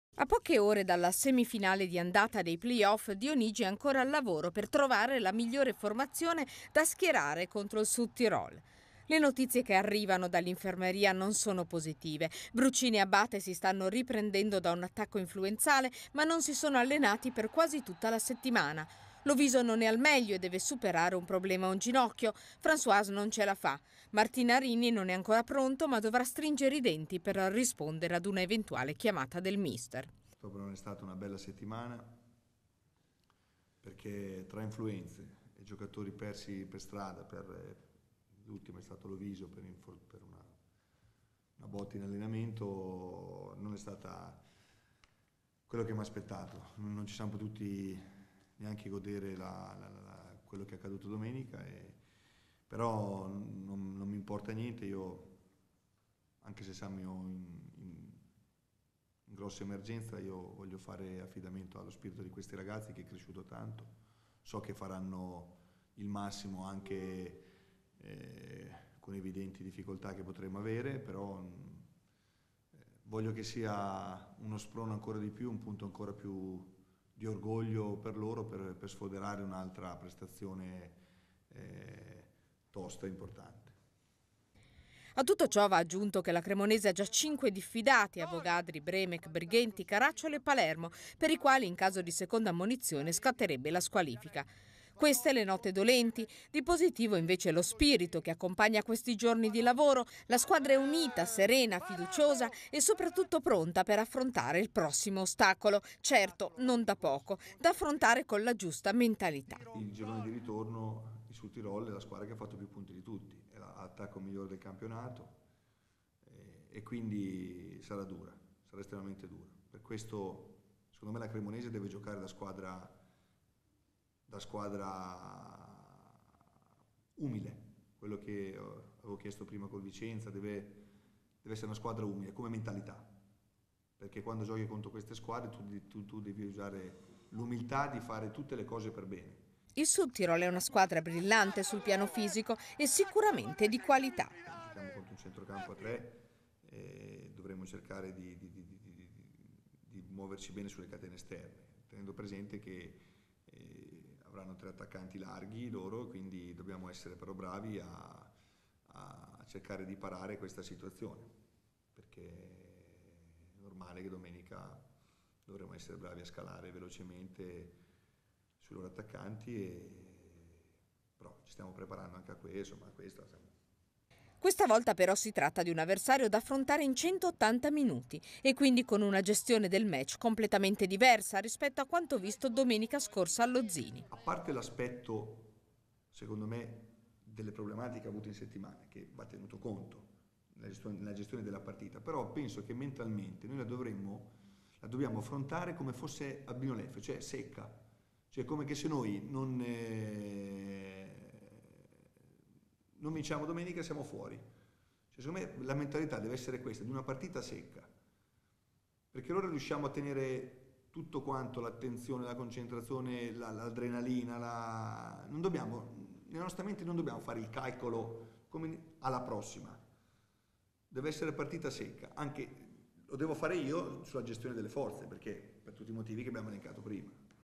Davide Dionigi, conferenza pre Sudtirol
Domenica semifinale di andata dei Playoff di Lega Pro